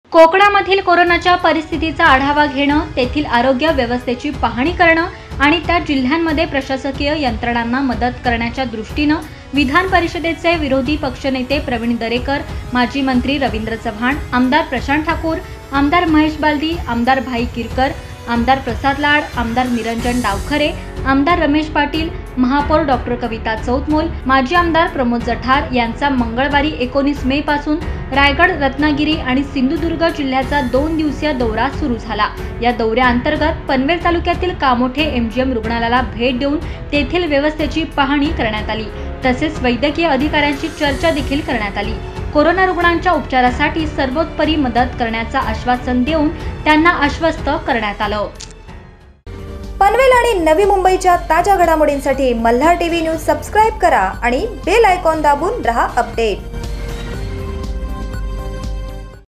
पनवेल तालुक्यातील एम जी एम रुग्णालयाला भेट देत वैद्यकीय अधिकाऱ्यांशी केली चर्चा !
malhartvnews at panvel | विधानपरिषदेचे विरोधी पक्ष नेते प्रविण दरेकर, माजी मंत्री रविंद्र चव्हाण, आमदार प्रशांत ठाकूर,आमदार महेश बालदी,महापौर डॉक्टर कविता चौतमोल यांच्या सह कोकणातील आमदारांनी पनवेल तालुक्यातील एम जी एम रुग्णालयाला भेट देत वैद्यकीय अधिकाऱ्यांशी केली चर्चा !